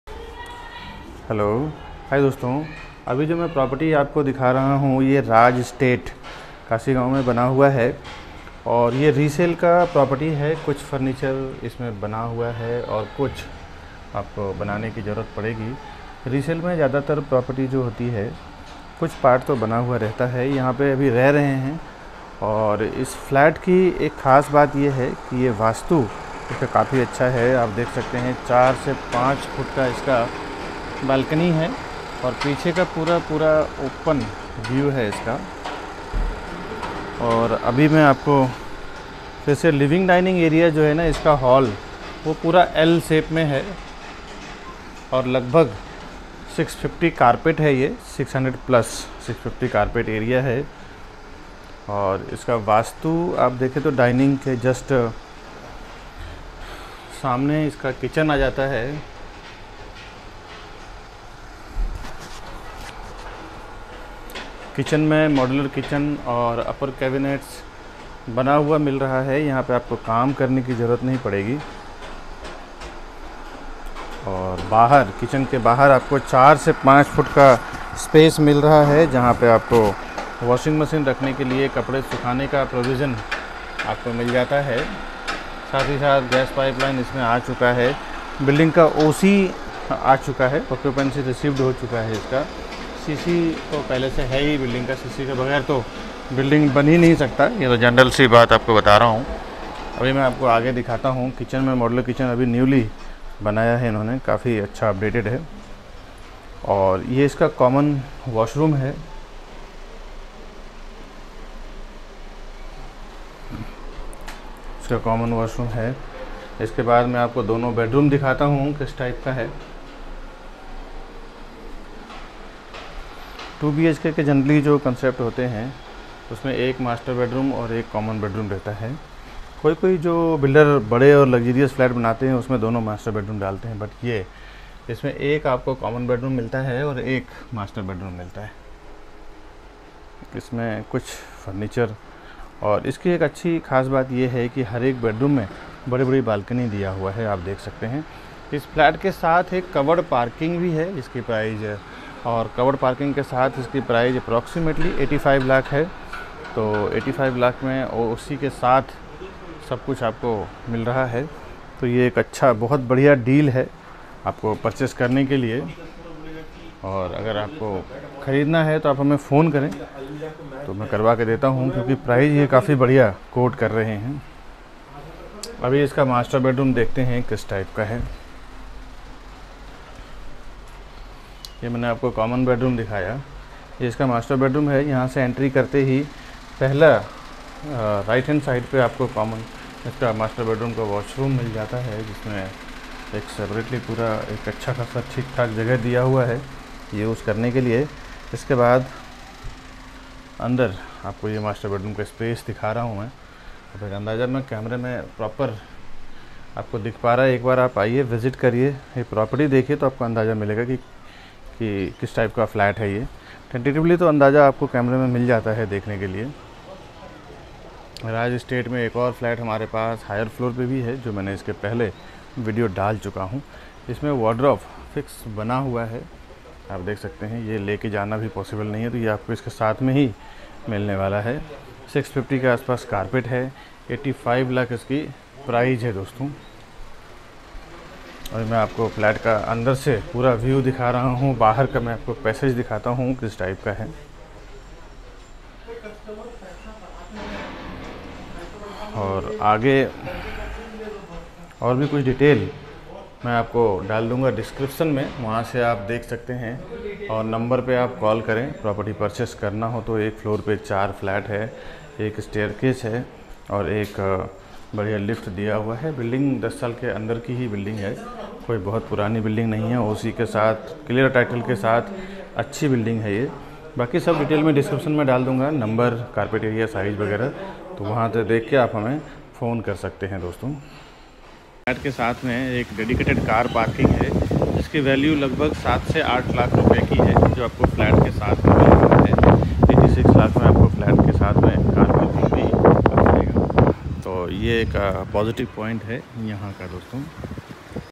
RAJ ESTATE MIRA ROAD/2BHK/85 LAKHS WITH OC AND PARKING/650 SQ.FT APPRX/PROPERTY PLAZA BIKASH BARNWAL
2BHK
CARPET AREA :- 650 SQ.FT APPROX
PRICE:- 85 LAKH NEGOTIABLE